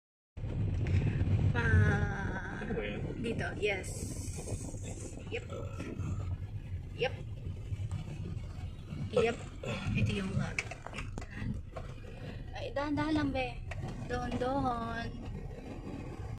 Lot for Sale in Vista Verde Cainta near Sta Lucia
Phase 8
Cash only
252sqm
11,500 pesos per sqm (no discount)